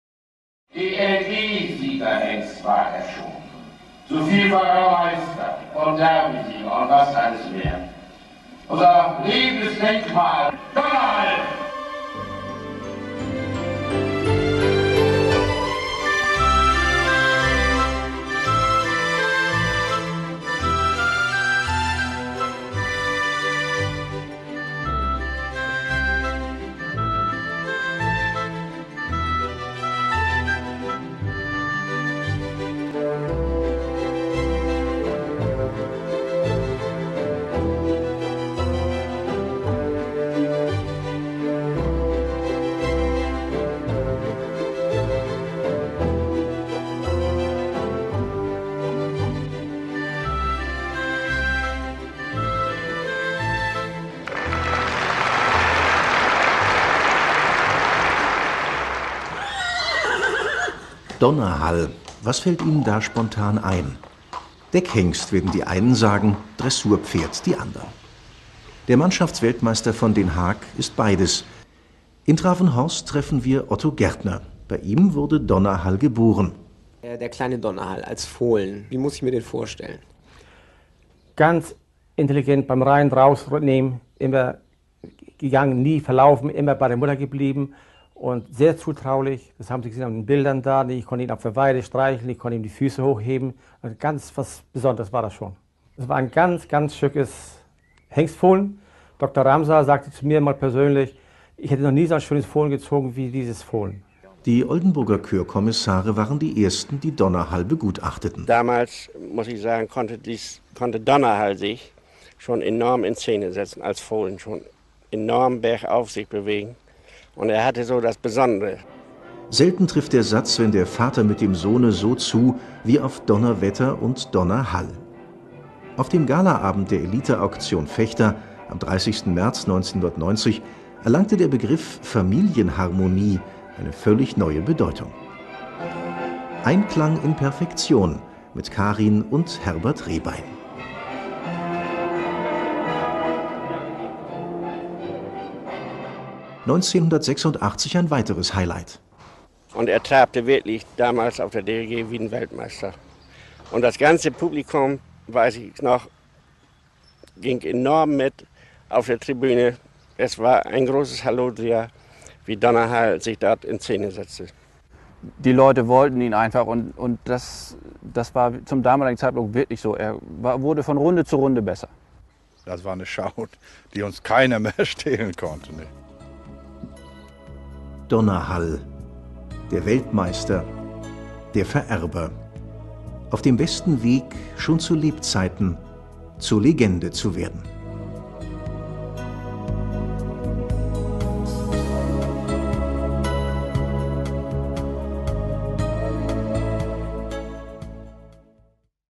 Donnerhall - ein Denkmal ganz privat
Donnerhall - der Dressurstar und Zuchthengst war bereits zu Lebenszeiten eine Legende. Kein anderer Hengst hat die Verbindung von Zucht und Sport jemals so verkörpert wie Donnerhall. Hier ein Ausschnitt aus seinem Videoportrait von 1997, Konzeption: Jan Tönjes, Kamera und Schnitt: Thomas Vogel

This video gives an insight into the most successful dressage horse in 1994, the marvellous chestnut stallion Donnerhall. Interviews, action footage and pitures show the unique career of this exceptional equestrian personality.